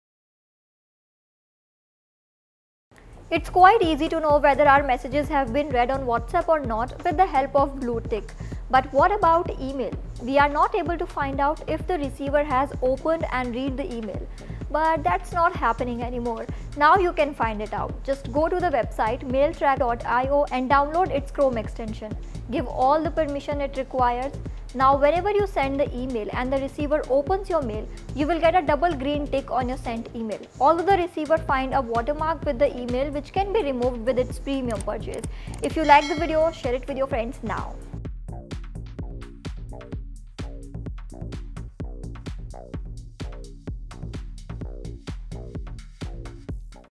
How To See If Someone Read Your Email | Gmail Read Receipt | Mail Tracker For Gmail
Depending on the situation, it can be super helpful to see when important emails are read. Using an email tracking system or a return receipt in Gmail can let you know whether or not your message has been opened. When you get notifications in real-time, you can respond appropriately with the next step and at the right moment.